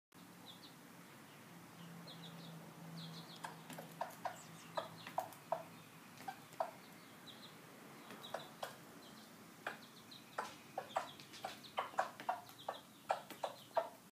Breakfast time for Mama Hen and her chicks
Baby chickens eating in my kitchen